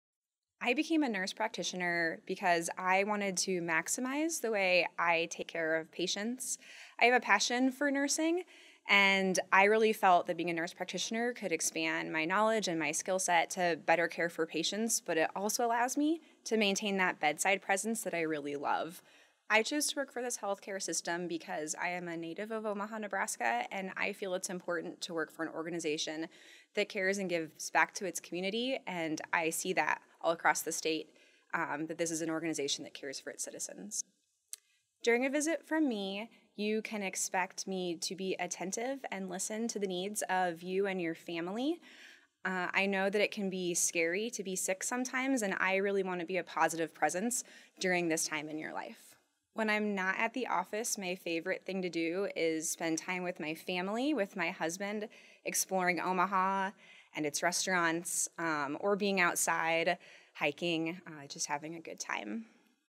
Kaitlin Maasdam, DNP - Pulmonology
Kaitlin Maasdam, DNP is a nurse practitioner in pulmonology at CHI Health.